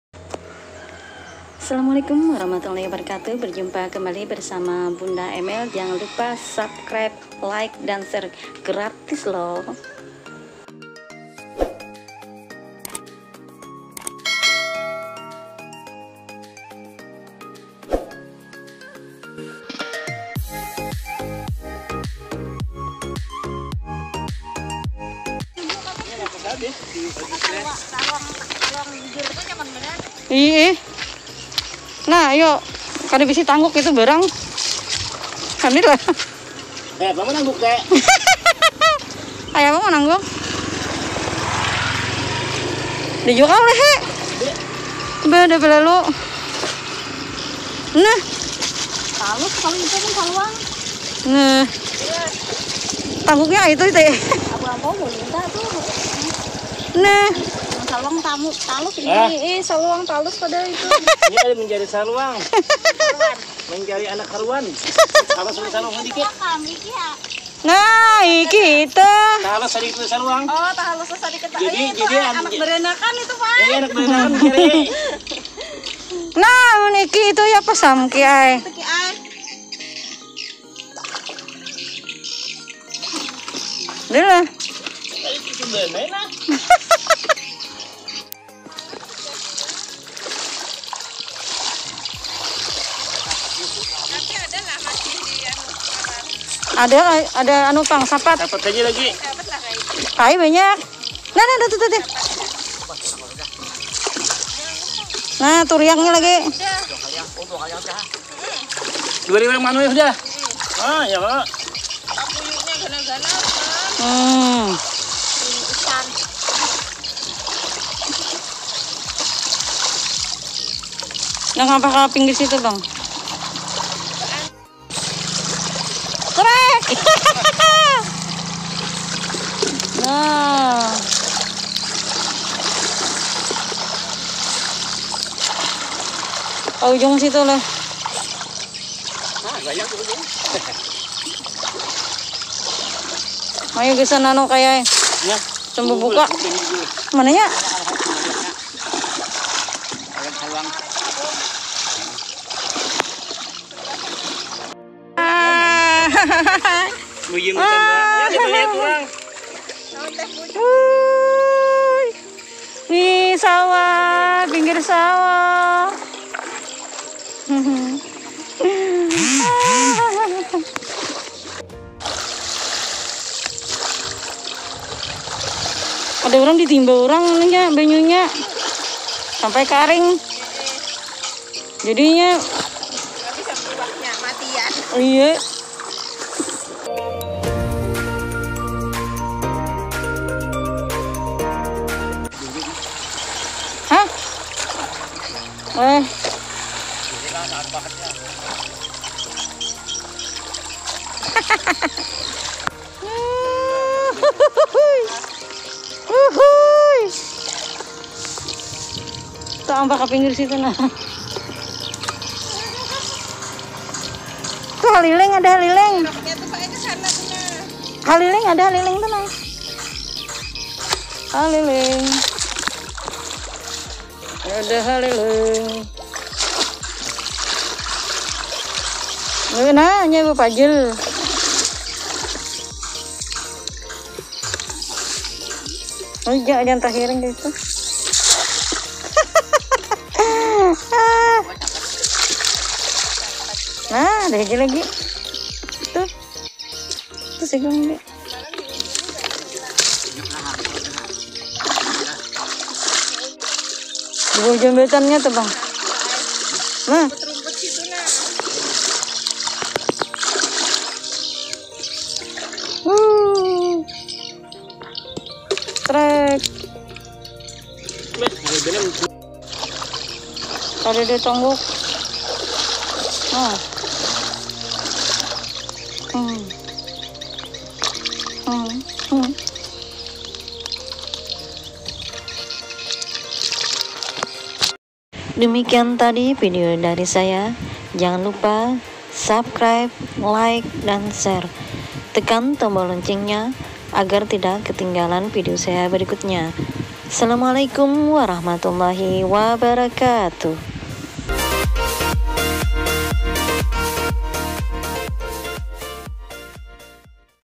Mencari Ikan Di Sawah Dapat Ikan Sepat
Selamat menjalankan Ibadah Puasa Ramadhan.
Berjumpa kembali bersama Bunda Emil.
Video kali ini "Mencari Ikan Di Sawah Dapat Ikan Sepat"